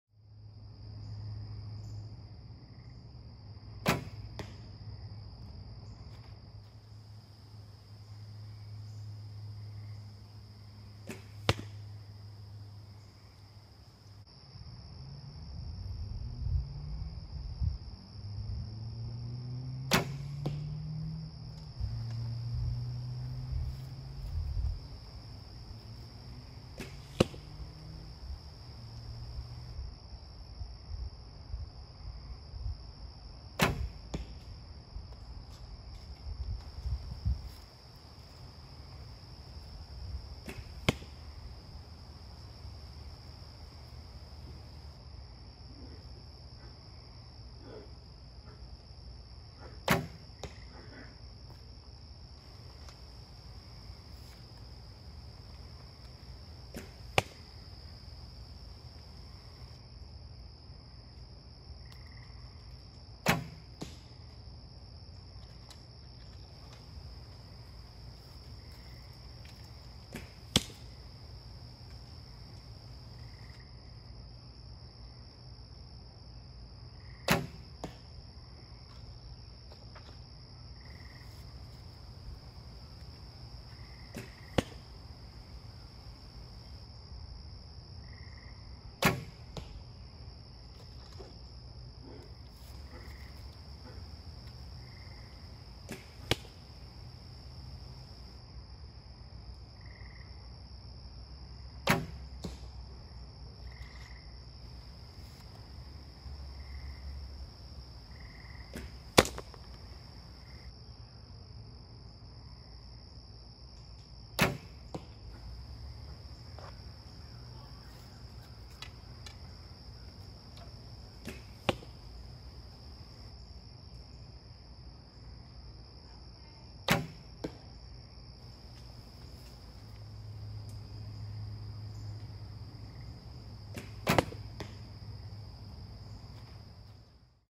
Bear Grizzly Takedown
Shooting a modified Bear Grizzly turned into a takedown bow.